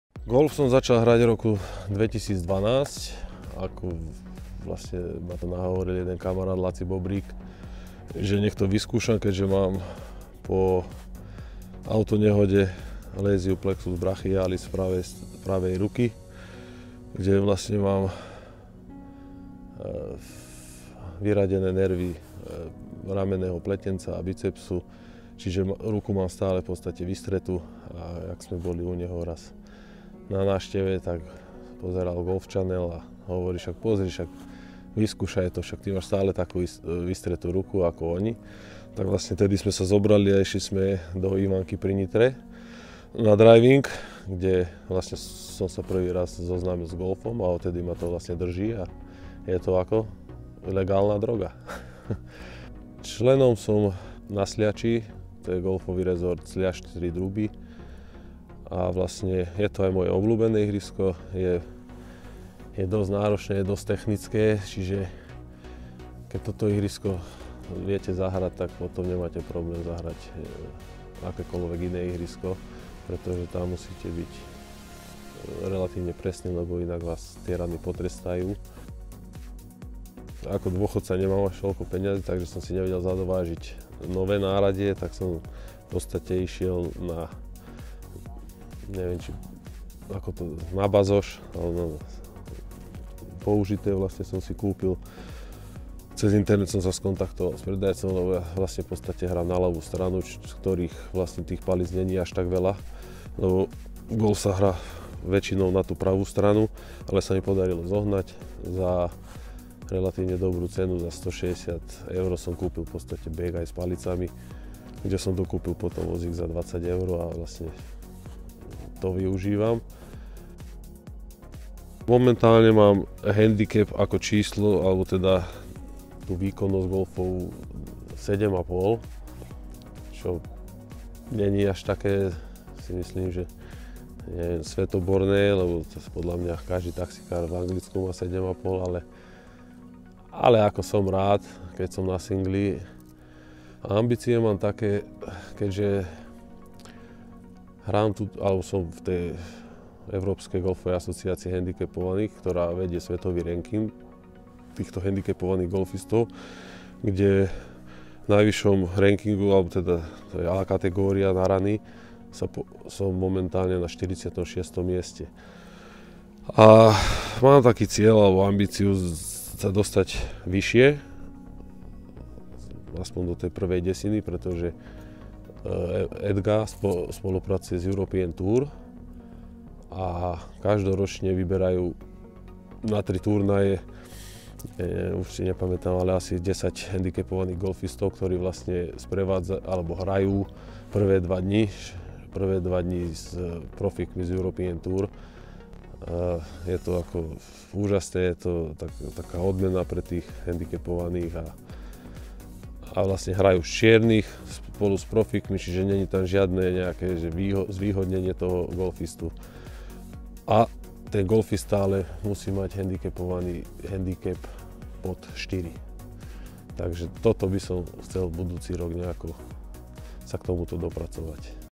Jaroslav Šalaváry - Golf je ako legálna droga
⛳ Navštívili sme Golfový klub Agama v Koši, kde majú naši paragolfisti vytvorené ideálne podmienky na trénovanie.

🏌‍♂ Jaroslav Šalaváry patrí v rámci našich hendikepovaných golfistov medzi širšiu svetová špičku. So singlovým hendikepom 7,5 je jasným dôkazom, že golf sa dá hrať za každých okolností bez rozdielu.

➡ S golfom sa prvýkrát zoznámil vďaka kamarátovi, ktorý ho vzal na driving v Ivanke pri Nitre. Hovorí, že golf je ako legálna droga a je to šport na celý život. Jeho golfovým snom je dostať sa do prvej desiatky v rámci rankingu európskej golfovej asociácie hendikepovaných.